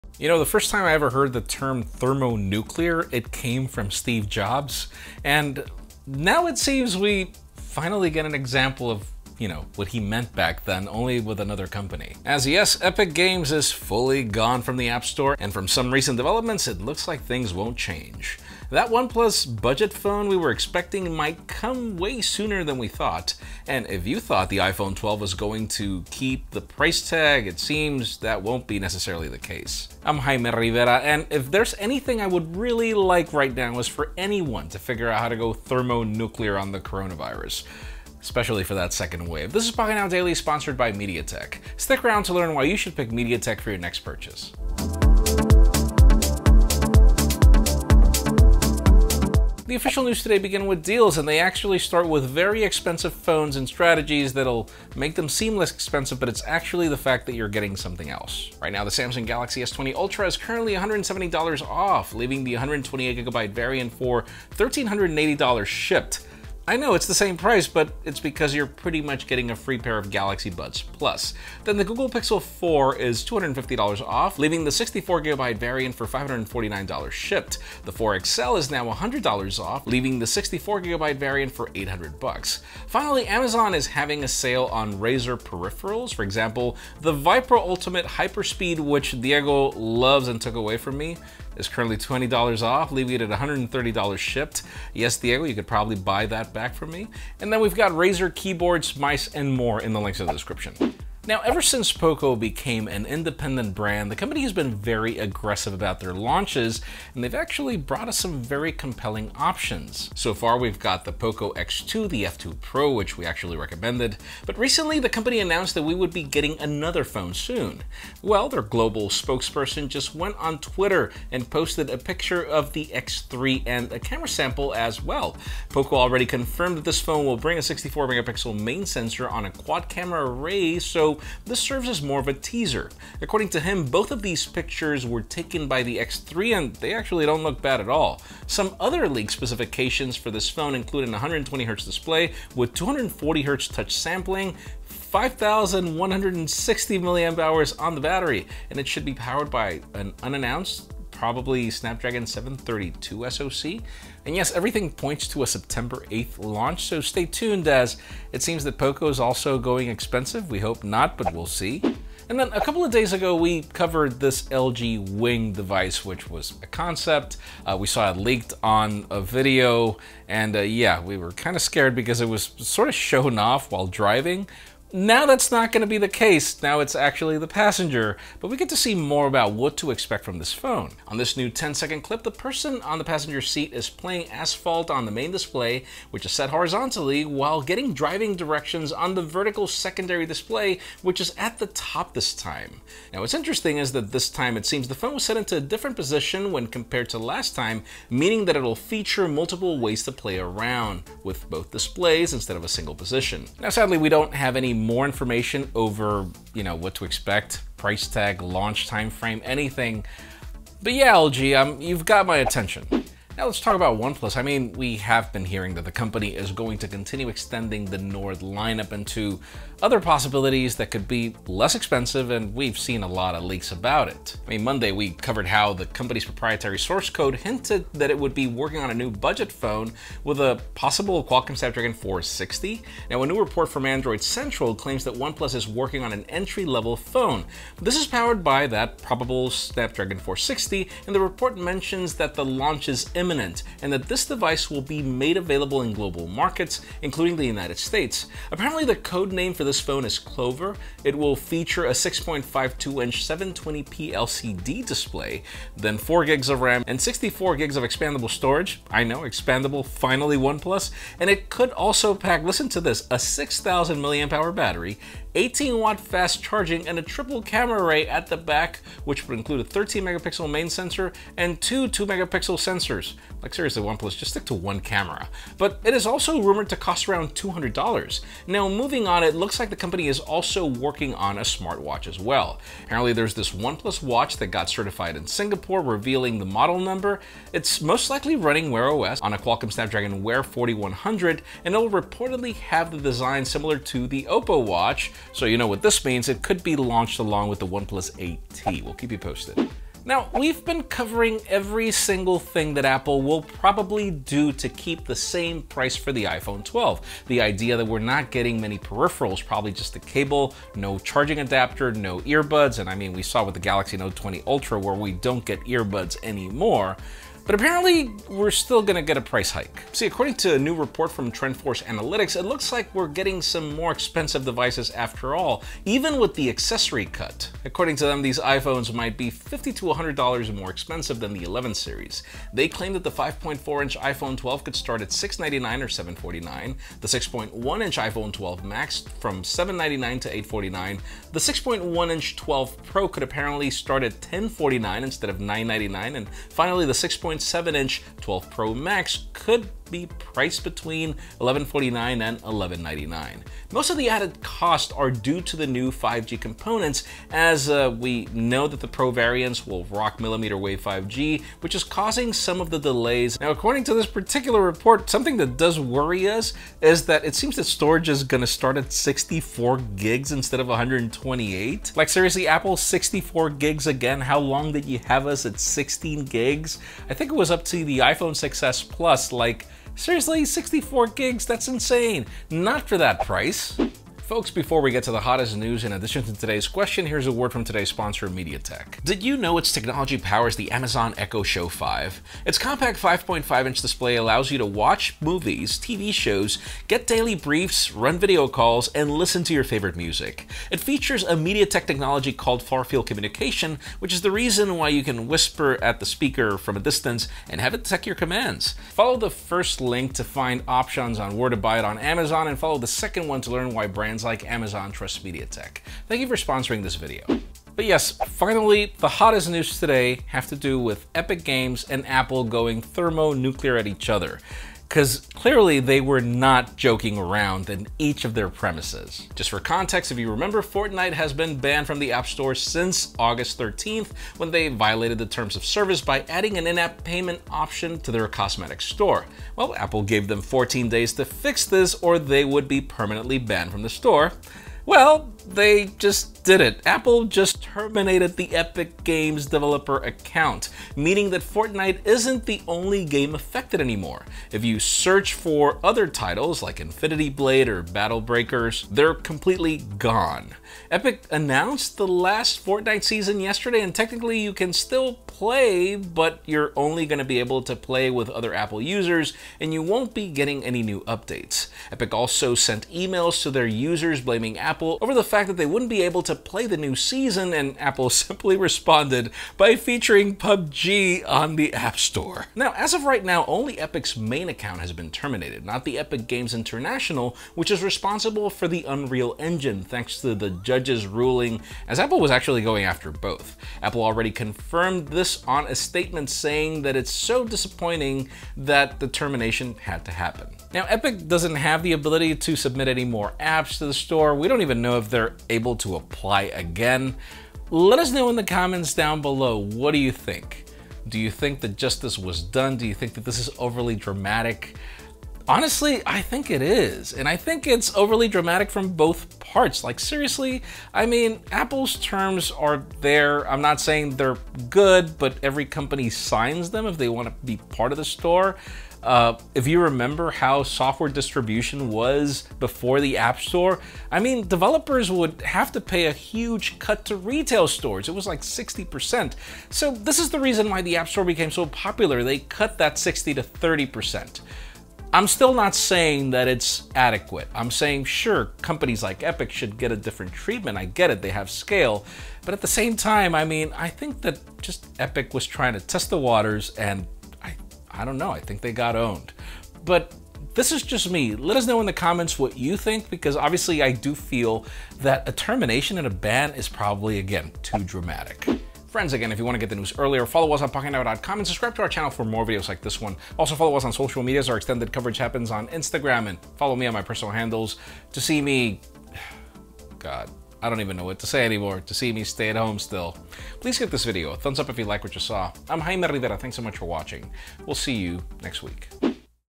Apple vs Epic Games: Game Over!
- Amazon Echo Show 5 on sale:

On Pocketnow Daily, we have some deals on the Samsung Galaxy S20 Ultra, the Pixel 4 and Razer peripherals. It looks like the POCO X3 is coming very soon as the company is leaking it. We have another leaked video of the LG Wing concept’s gaming capabilities. A new OnePlus Nord and a OnePlus Watch might be coming very soon. It looks like even with all the changes, the iPhone 12 might still be getting a price hike. Finally, Apple just ended Epic Games.. literally.

0:00 - Intro
0:50 - Today’s Deals:
- Samsung Galaxy S20 Ultra
- Google Pixel 4
- Google Pixel 4 XL
- Razer Viper Ultimate
- Razer Ornata Chroma
- Razer Blade 15
- More Razer Deals

News:
1:42 - POCO X3 will feature a 64MP primary camera; samples revealed
2:43 - LG’s Wing smartphone with a rotating secondary display appears in another leaked video
3:34 - A budget OnePlus ‘Clover’ with Snapdragon 460 SoC, 6000mAh battery is coming soon
- OnePlus could be working on a smartwatch, might launch it by year-end
5:00 - The prices of the iPhone 12 lineup will apparently go up
6:59 - Apple has terminated Epic Games’ developer accounts